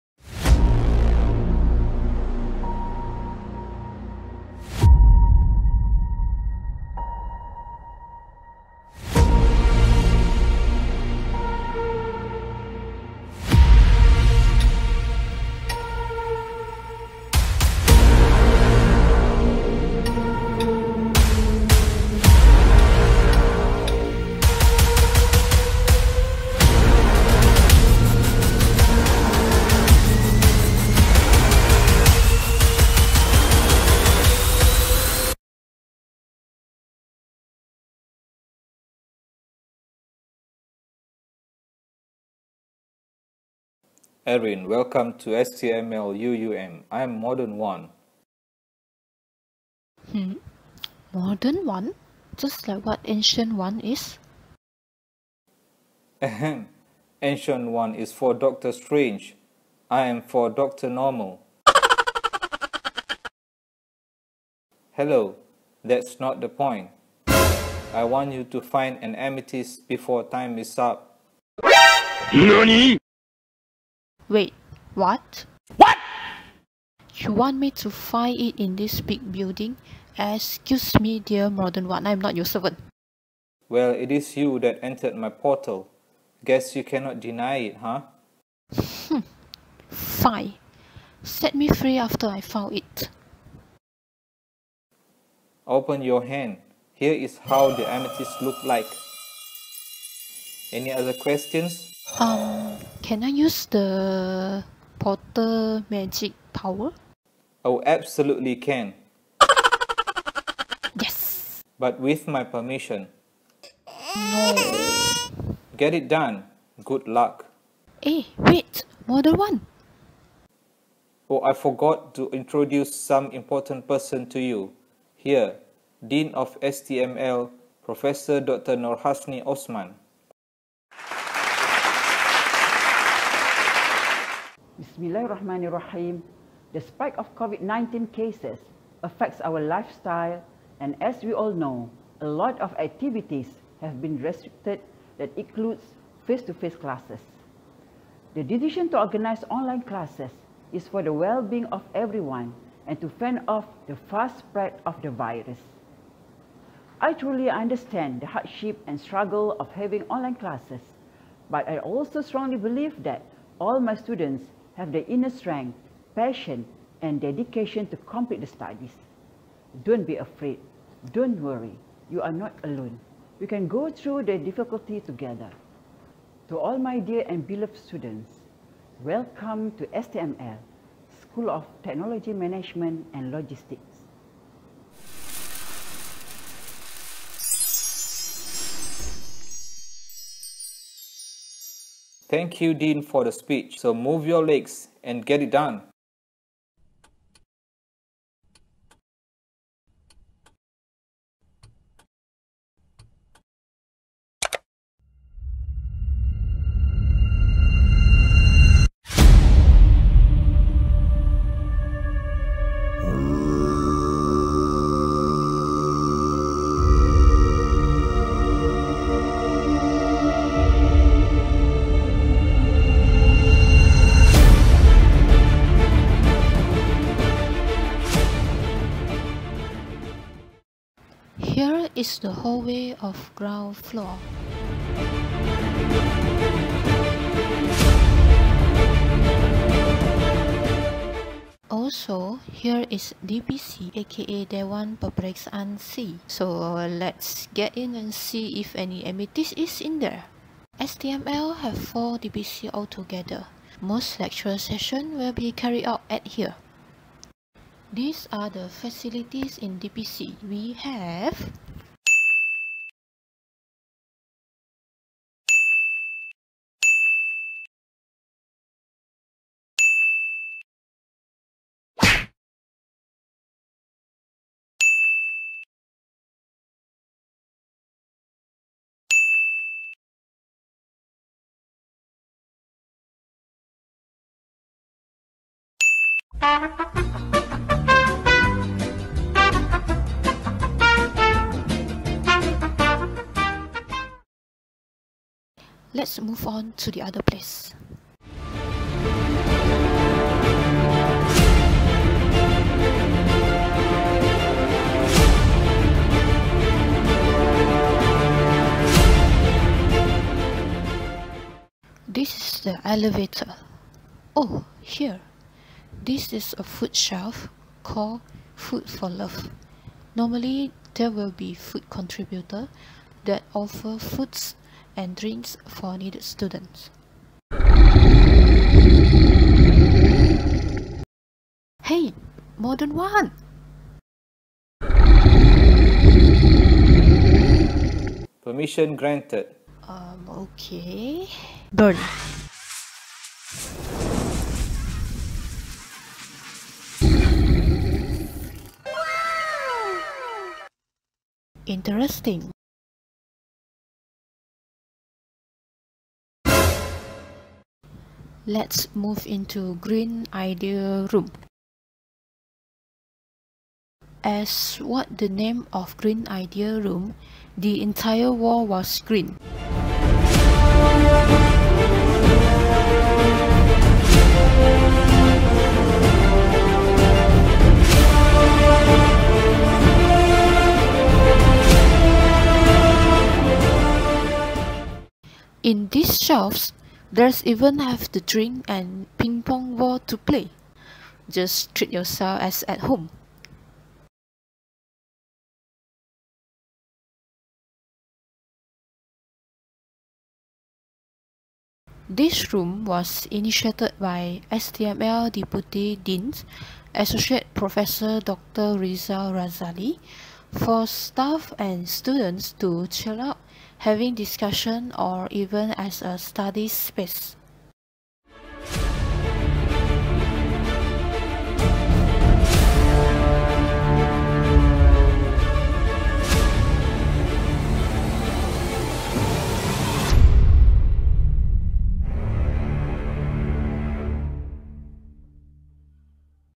PART 2 CHOSEN VS CURSED
After Eryn entered the portal, she saw a building of Pusat Pengajian Pengurusan Teknologi dan Logistik followed by the mysterious voice from Modern One. Modern One instructed Eryn on a mission to find an Amethyst in the building. In this Part, apart from searching for the Amethyst, Eryn will introduce the facilities provided in STML and fight off the mutants and aliens who are also hunting for the Amethyst.